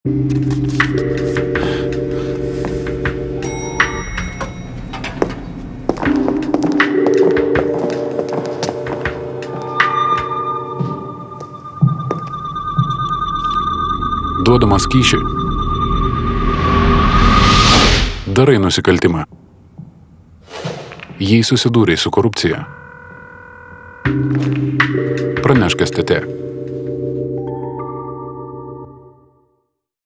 Specialiųjų tyrimų tarnyba (STT) socialinė antikorupcinė reklama
Specialiųjų tyrimų tarnybos socialinė antikorupcinė reklama. Duodamas kyšį, darai nusikaltimą. Jei susidūrei su korupcija  pranešk STT. „Karštoji linija visą parą (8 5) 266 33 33